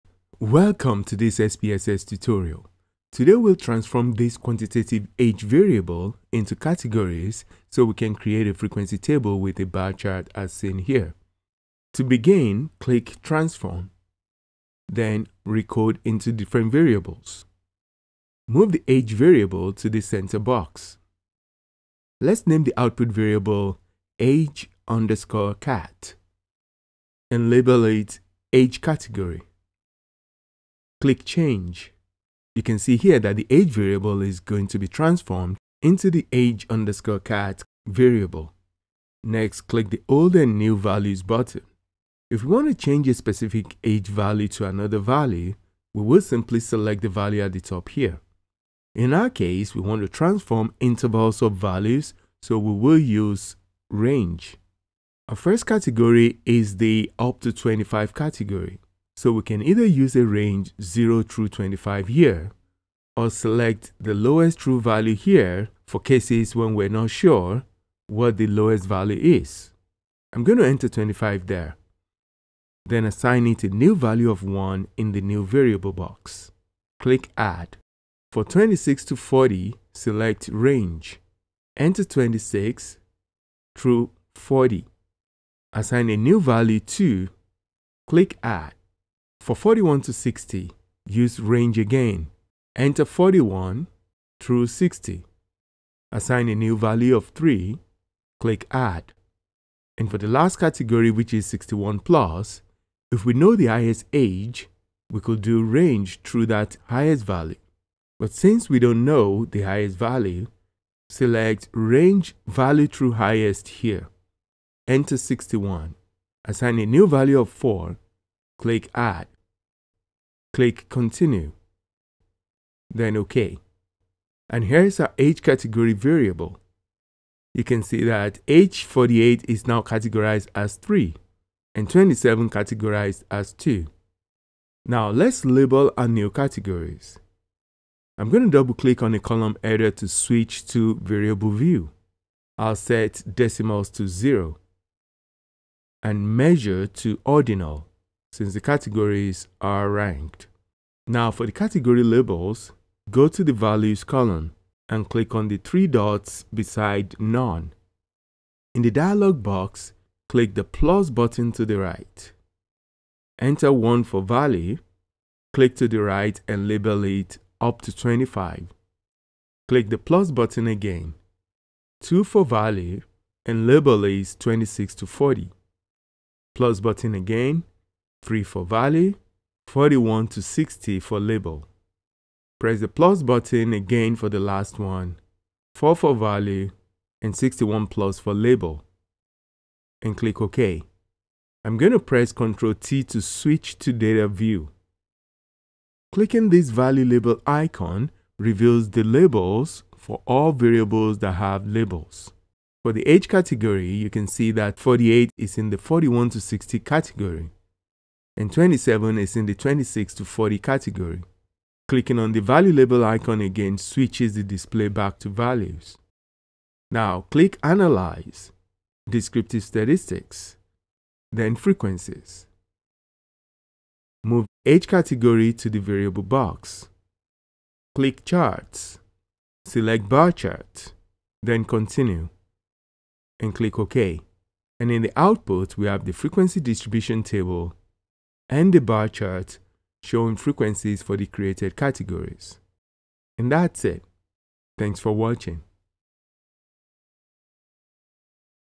SPSS - Recoding Age into Categories and Creating Frequency Charts - Recoding into different variable
Learn how to transform a continuous variable like age into categorical groups and create informative frequency tables and bar charts in SPSS. This tutorial provides clear instructions and visual examples.